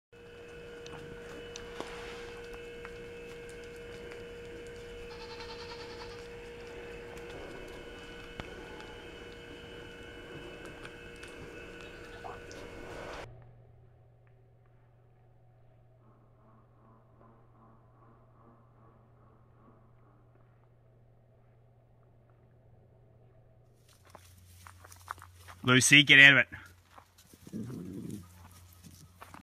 Cool Goat Loves To Lick Salt Wheel (Storyful, Wild Animals)
This Tennessee goat isn't too worried about watching her levels of salt intake. She can’t get enough of licking this salt wheel and just keeps going. It certainly helps that she also looks cool while wearing some sunglasses and a knitted hat!